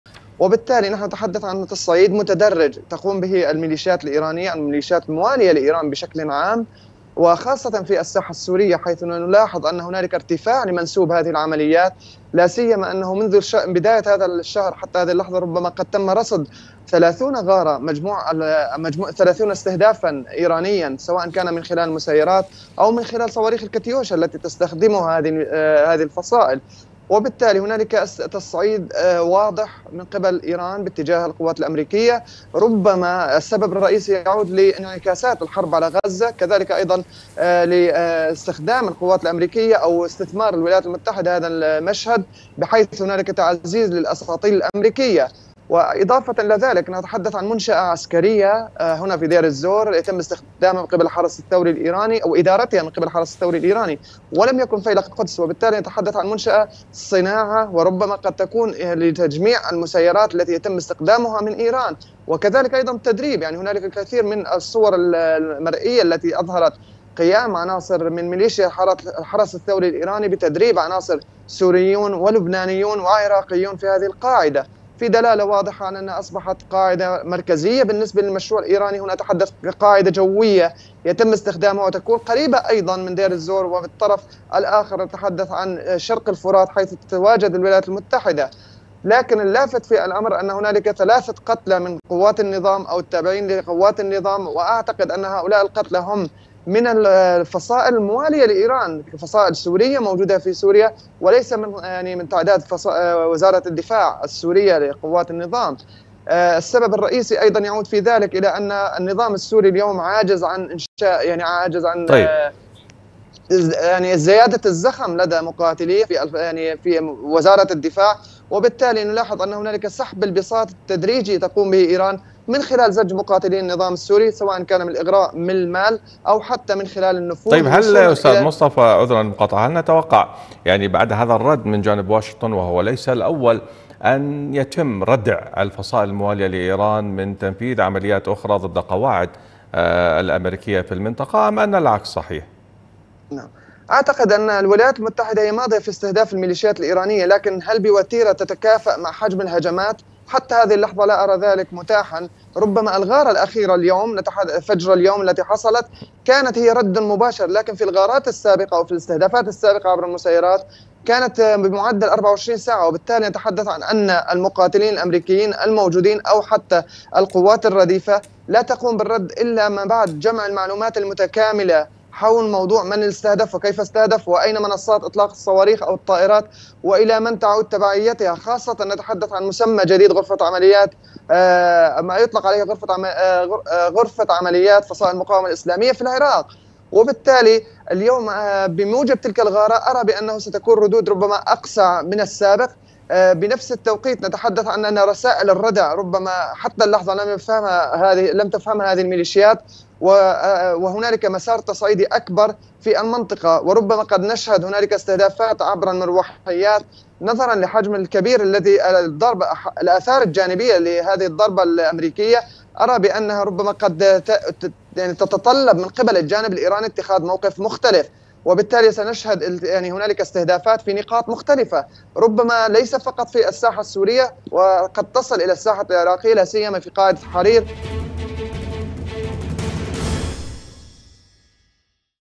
اذرع ايران تصعد من هجماتها في العراق وسوريا .. وهكذا ردت امريكا
موقع قناة التغيير الفضائية  الالكتروني